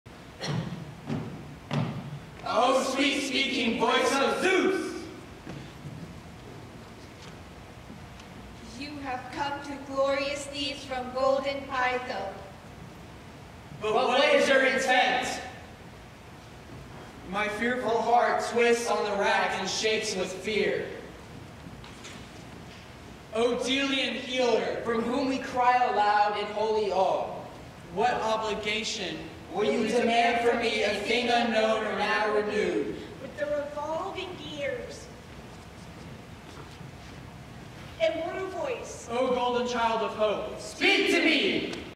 Greek Ode - One
A Greek Ode as presented by the Drama class in September 2023.